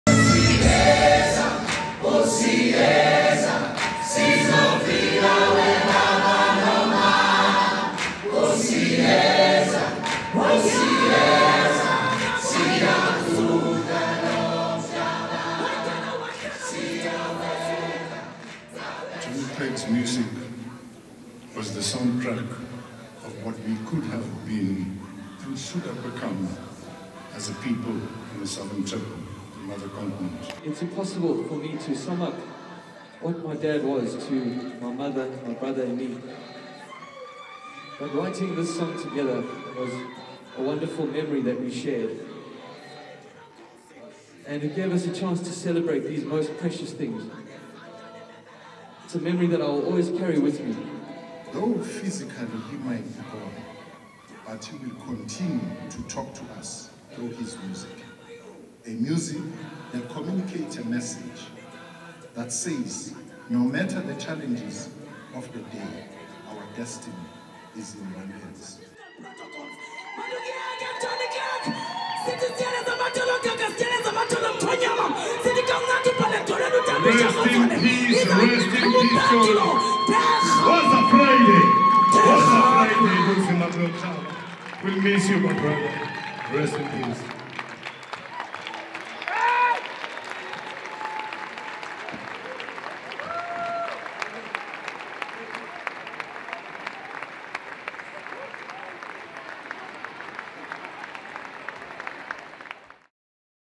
'Clegg's music was the soundtrack of what we could have been as a nation '
A memorial service filled with music, nostalgia and tributes was held for the Johnny Clegg at Sandton Convention Centre on 26 July. Several artists joined together on stage in song to honour the musician.